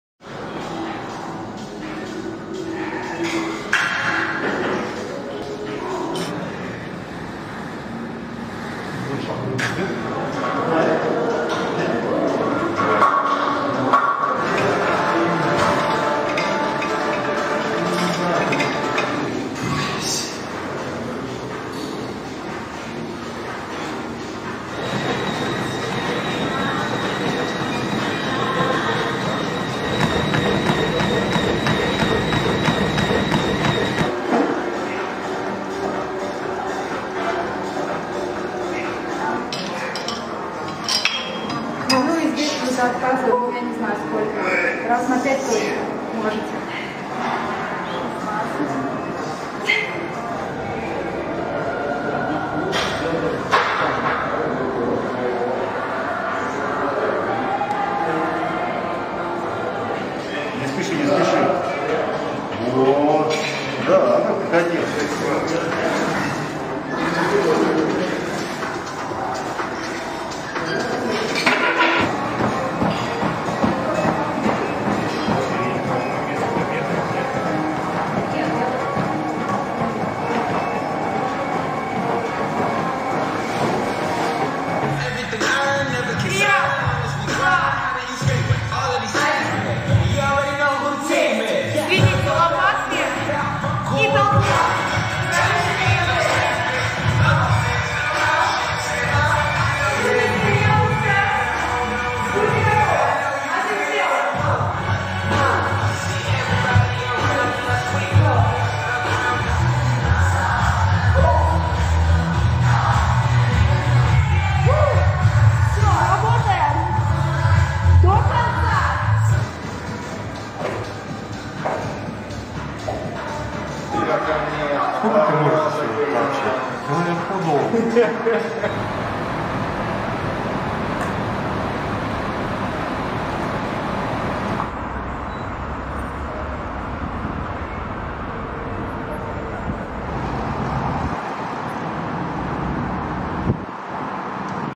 Фитнес-клуб "Спортстар" в Москве
Россия. Москва. Посетители фитнес-клуба "Спортстар". В связи с распространением коронавируса, посещение фитнес-центров возможно только при соблюдении антиковидных ограничений.

Спасибо всём за поддержку моего канала!